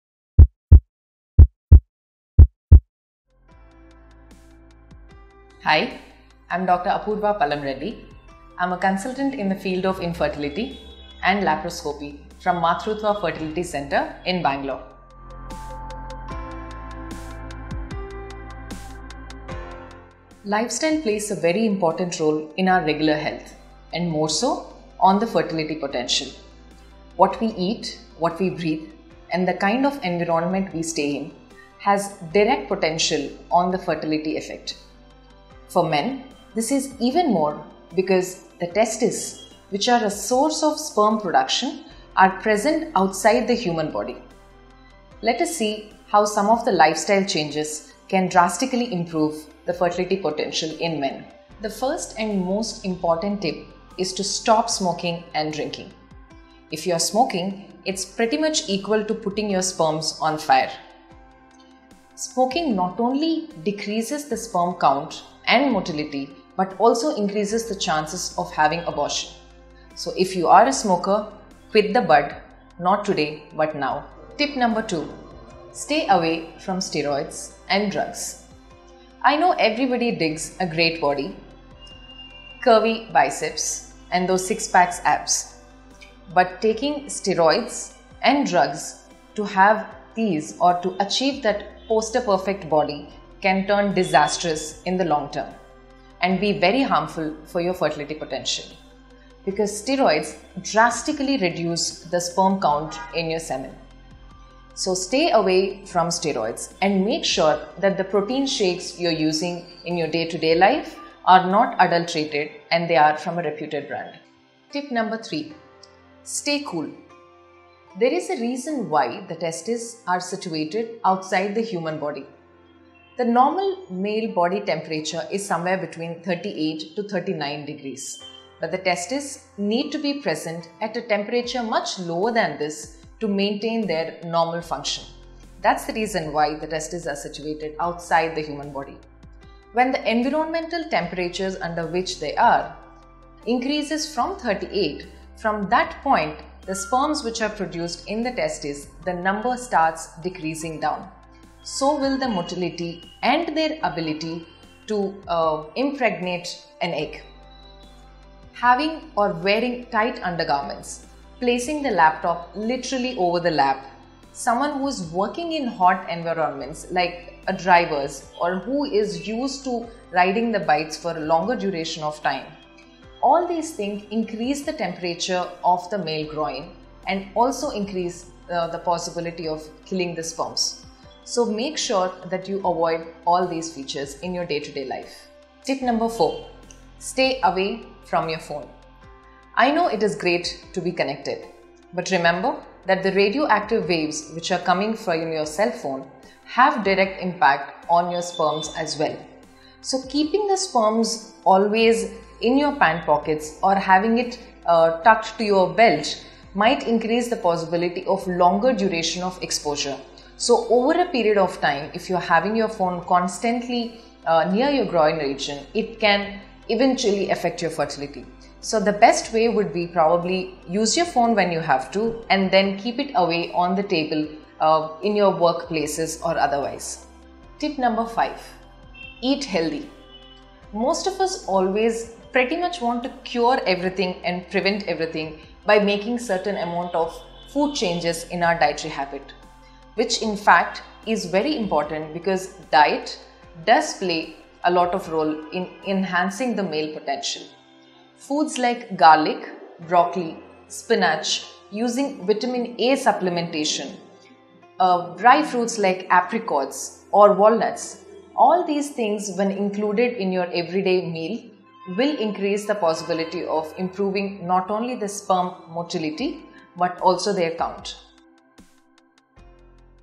How does a change in lifestyle improve male fertility? - Dr. Apoorva P Reddy
Lifestyle plays a very important role in our regular health and also on fertility potential. The first thing is to stop smoking you are smoking it is pretty much equal to putting your sperms on fire. Smoking not only decreases the sperm count and motility, but also increases the chances of abortion. So stay away from smoking. Tip No 2. Stay away from steroids and drugs. So taking steroids and drugs for the poster perfect body can turn disastrous in the long run and can present with fertility related issues because steroids reduce the sperm count. Tip 3. Stay cool. There is a reason why the testis is situated outside the human body. The normal male body temperature is somewhere between 38 – 39 degree centigrade. But the testis needs to be at a much lower temperature to maintain their normal function. That is the reason why the testis is situated outside the human body. Wearing tight undergarments, placing the laptop literally on the lap, someone who is working outside in hot temperatures lie drivers or who is used to driving the bike for a longer duration of time, all these will increase the temperature of the male groin and also increase the possibility of killing the sperms. So avoid all these features in your day to day life. Tip No4. Stay away from your phone. The radioactive rays coming from the devices have a direct effect on your sperms. So if you are having your phone constantly over a longer period of time near your grin region, it can eventually affect your fertitlity. So the best way is use your phone when you have to and the keep it  away from your body, on the table in your work places. tip no 5, Eat healthy food. Food like garlic, spinach, using Vitamin A supplementation, dry fruits like apricots and walnuts, all these when included in an everyday meal, increases the possibility of improving the sperm motility but also the count.